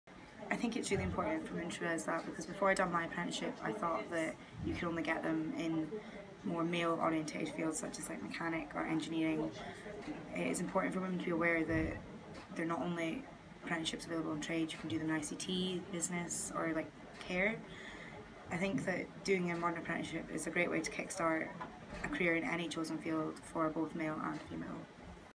Congratulations Shantelle!
Aberdeenshire Council Apprentice of the Year 2018 Shantelle Birdi talks about how apprenticeships are for women as well as men. A timely topic as today (Thursday, March 8 2018) is International Women's Day.